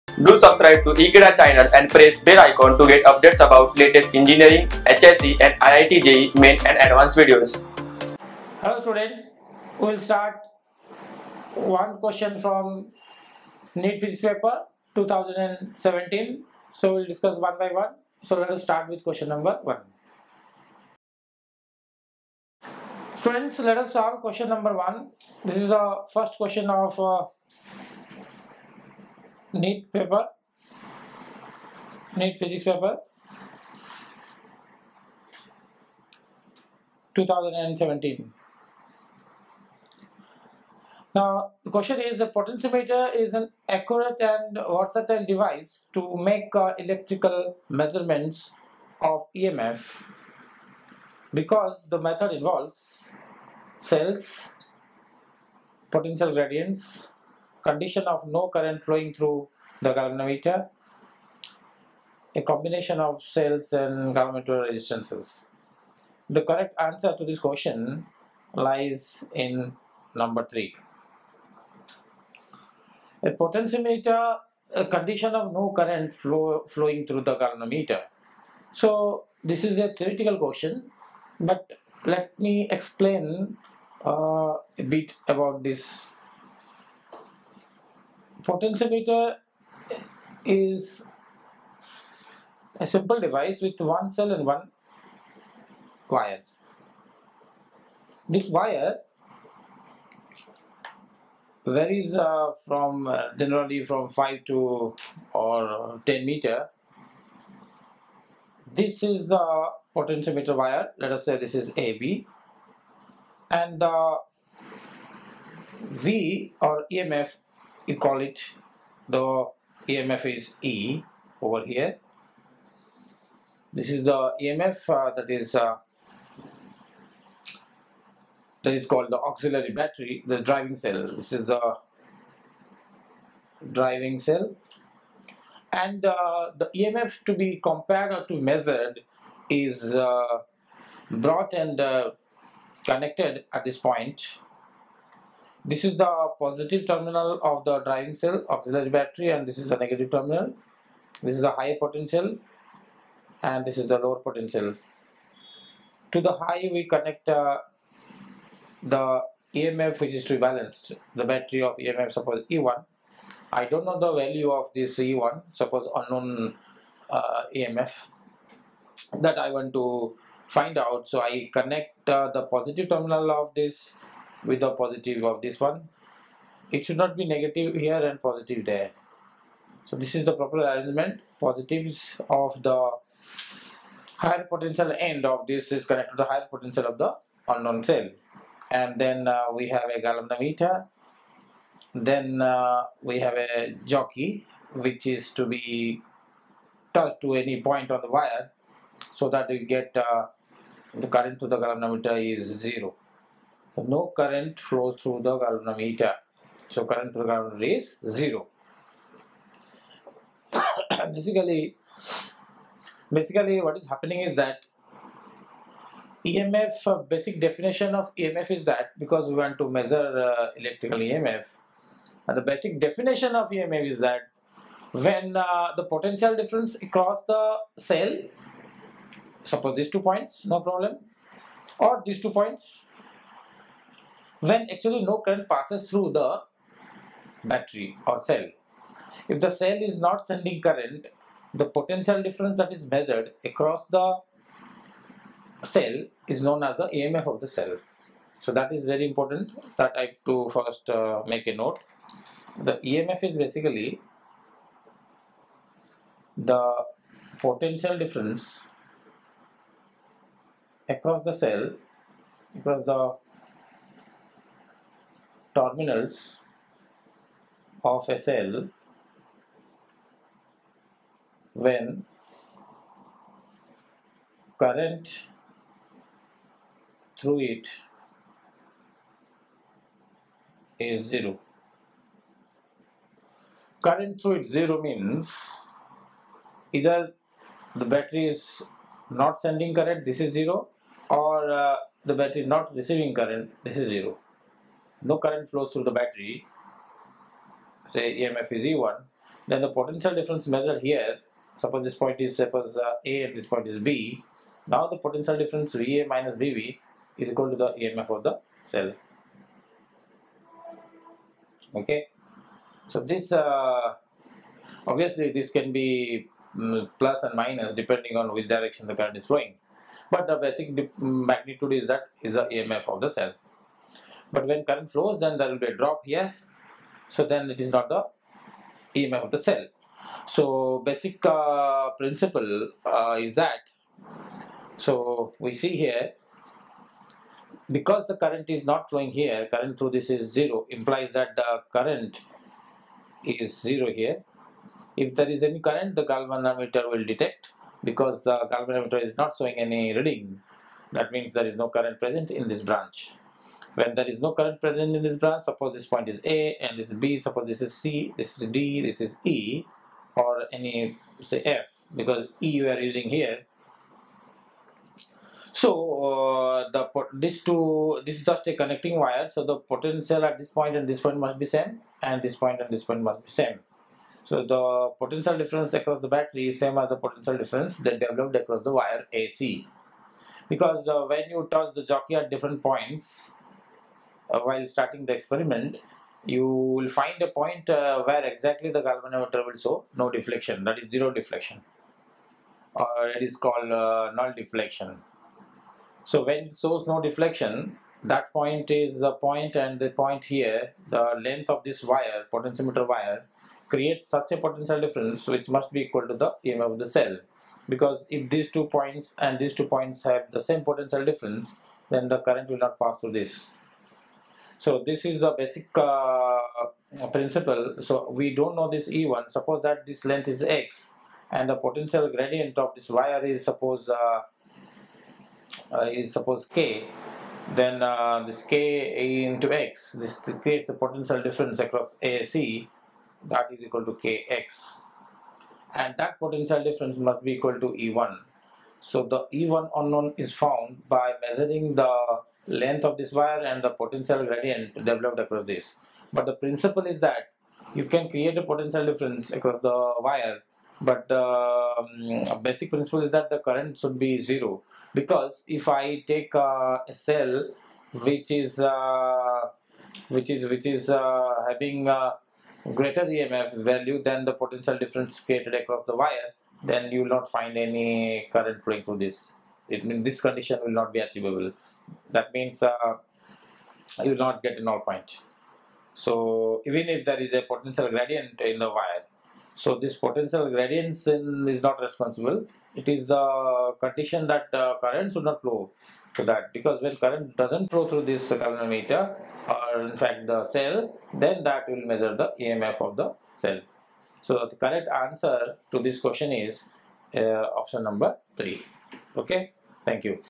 NEET 2017 Physics Paper Solution - Question 1 - NEET Physics Paper Video Solution
Video Lecture on Question 1 from NEET 2017 Physics Paper Solution of NEET Physics Paper Video Solution for HSC, CBSE & NEET Students.

Happy Learning : )